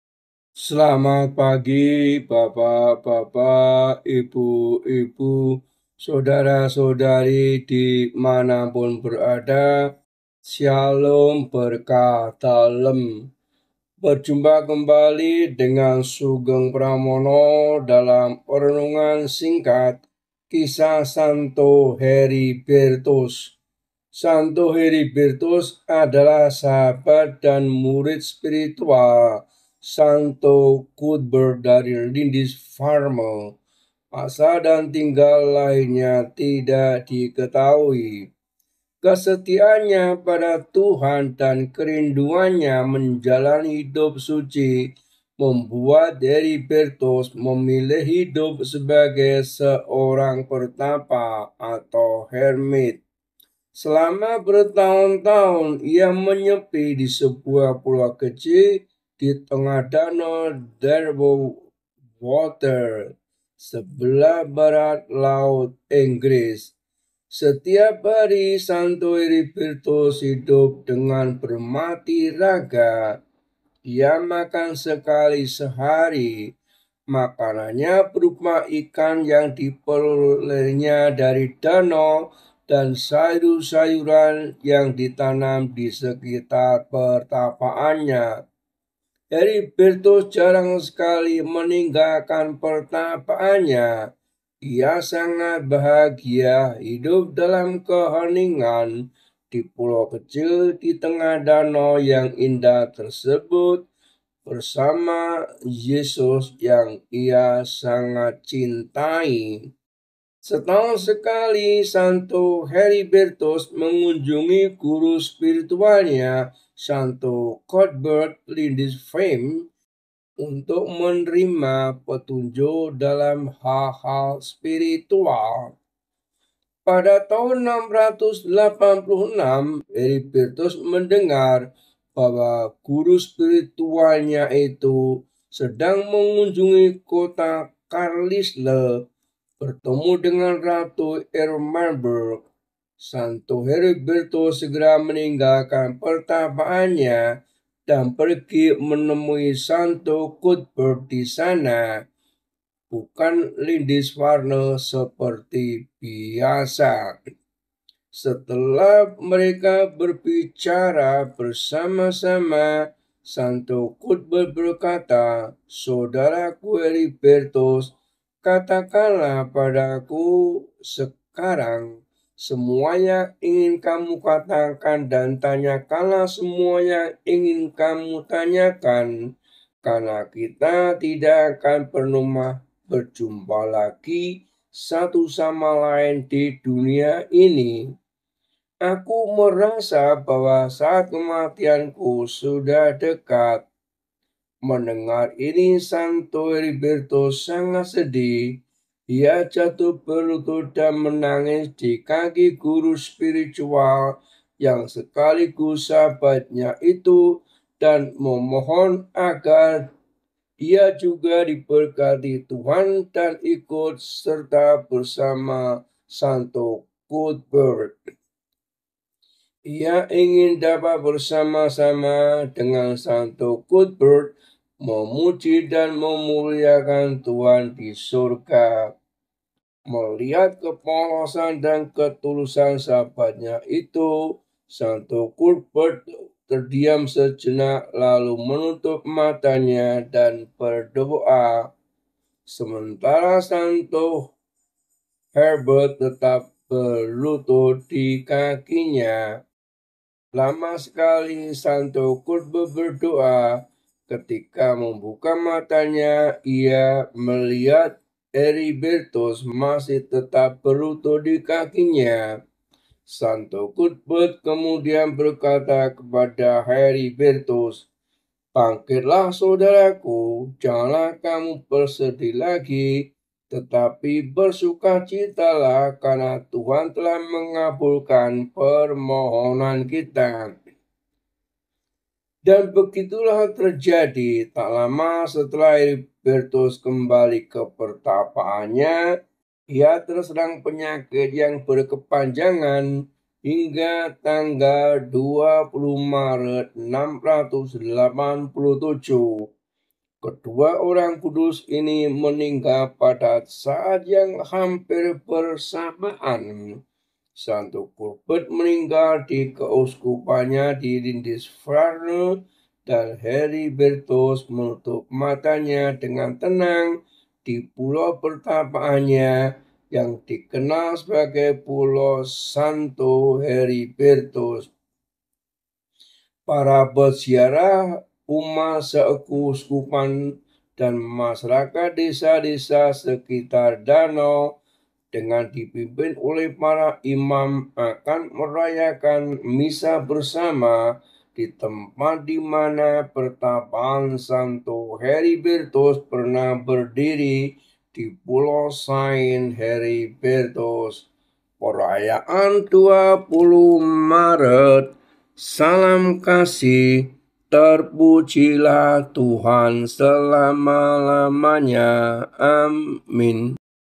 Santo Heribertus | 20 Maret 24 | Sugeng Pramono
SANTO  HERIBERTUS

Heribertus dari Derwentwater, The Hermit of Derwentwater, Pertapa dari Derwentwater

St. Heribertus adalah sahabat  dan murid spiritual dari St. Cuthbert dari Lindisfarne.  Asal dan tanggal lahirnya tidak diketahui.

Kecintaannya pada Tuhan dan kerinduannya untuk menjalani hidup yang suci membuat Heribertus memilih untuk hidup sebagai seorang pertapa (hermit). Selama bertahun-tahun ia menyepi di sebuah pulau kecil  di tengah Danau Derwentwater  disebelah Barat Laut Inggris.

Setiap hari St.Heribertus hidup dengan bermatiraga. Ia makan sekali sehari. Makanannya berupa ikan yang diperolehnya dari danau dan sayur-sayuran yang ditanam di sekitar pertapaannya.

Heribertus jarang sekali meninggalkan pertapaannya. Ia sangat berbahagia hidup dalam keheningan dipulau kecil ditengah danau yang indah tersebut bersama Yesus yang sangat ia cintai.

Setahun sekali St.Heribertus mengunjungi guru spiritualnya St.Cuthbert di Lindisfarne  untuk menerima petunjuk  dalam hal-hal spiritual.

Pada tahun 686,  Heribertus mendengar bahwa guru spiritualnya itu sedang mengunjungi kota Carlisle untuk bertemu dengan Ratu Eormenburg. St.Heribertus segera meninggalkan pertapaannya dan pergi menemui St.Cuthbert di sana, bukan di Lindisfarne seperti biasa.

Setelah mereka berbicara bersama-sama, St Cuthbert berkata, "Saudaraku Heribertus, katakanlah padaku sekarang semua yang ingin kamu katakan dan tanyakanlah semua yang ingin kamu tanyakan;  karena kita tidak akan pernah berjumpa lagi satu sama lain di dunia ini.  Aku merasa bahwa saat kematianku sudah dekat."

Mendengar ini St.Heribertus menjadi sangat sedih. Ia jatuh berlutut dan menangis dikaki guru spiritual yang sekaligus sahabatnya itu dan memohon agar ia juga diberkati Tuhan dan ikut serta bersama St.Cuthbert.  Ia ingin dapat bersama-sama dengan  St.Cuthbert memuji dan memuliakan Tuhan di surga.

Melihat kepolosan dan ketulusan sahabatnya itu, St.Cuthbert terdiam sejenak, lalu menutup matanya dan berdoa. Sementara St.Herbert tetap berlutut dikakinya.

Lama sekali St.Cuthbert berdoa. Ketika membuka matanya ia melihat Heribertus masih tetap berlutut dikakinya. St.Cuthbert kemudian berkata pada Heribertus; "Bangkitlah, saudaraku, janganlah kamu bersedih lagi, tetapi bersukacitalah karena Tuhan telah mengabulkan permohon kita."

Dan begitulah yang terjadi.  Tak lama setelah Heribertus kembali ke pertapaannya, ia terserang penyakit yang berkepanjangan,  hingga pada tanggal 20 Maret 687

kedua orang kudus ini meninggal pada saat yang hampir bersamaan.  St.Cuthbert meninggal di keuskupannya di Lindisfarne dan St.Heribertus menutup matanya dengan tenang di pulau pertapaannya yang kini dikenal sebagai Pulau St.Heribertus.

Pesta kedua Orang Kudus ini pun dirayakan pada hari yang sama yaitu pada setiap tanggal 20 maret.

Pada 1374, Uskup Carlisle, Thomas de Appleby, menjadikan pulau St.Heribertus sebagai tempat ziarah rohani, dan menganjurkan umat untuk berziarah ke pulau tersebut setahun sekali.  Tradisi ini masih dilajutkan sampai hari ini oleh umat di keuskupan tersebut.

Para peziarah bersama umat se-keuskupan dan masyarakat di desa-desa disekitar danau, dengan dipimpin oleh para imam akan merayakan Misa bersama di tempat dimana pertapaan St.Heribertus pernah berdiri;  di Pulau Saint Heribertus

Perayaan 20 Maret

Salam Peradaban Kasih : Sugeng Pramono